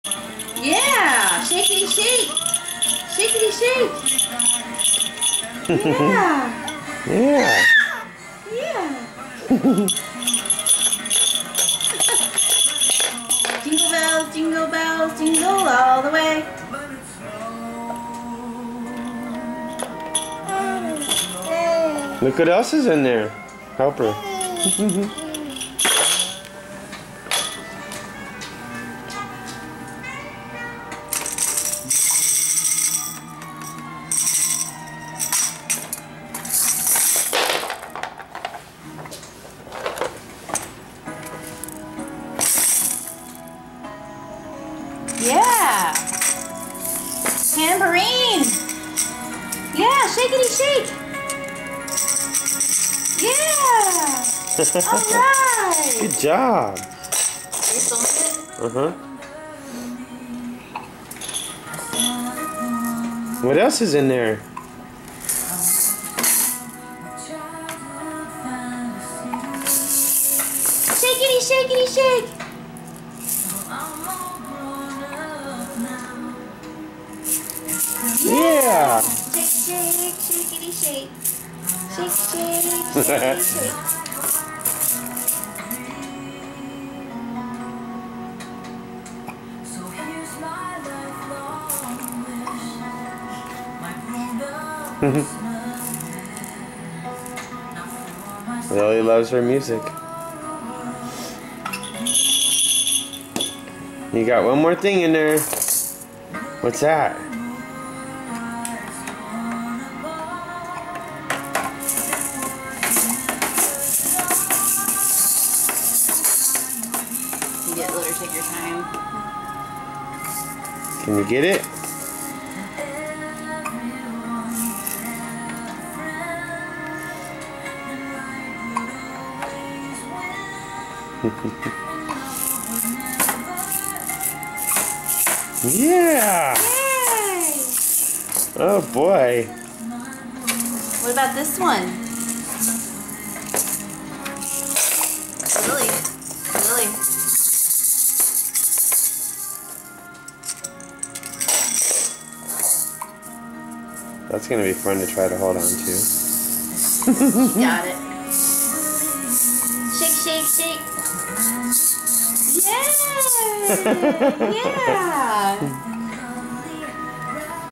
12-25-13 New Drum Set
Lily tries out her new B Parum Pum Pum Drum set that Santa brought for her.